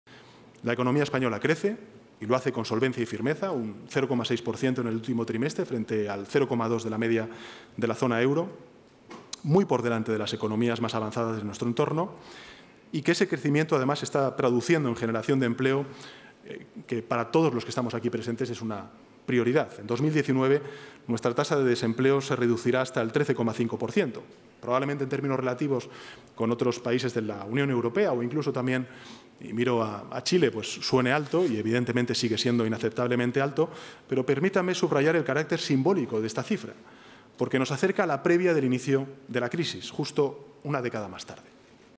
Sánchez dice que la economía española crece "con solvencia y firmeza"
El presidente del Gobierno, Pedro Sánchez, ha asegurado que "la economía española crece y lo hace con solvencia y firmeza".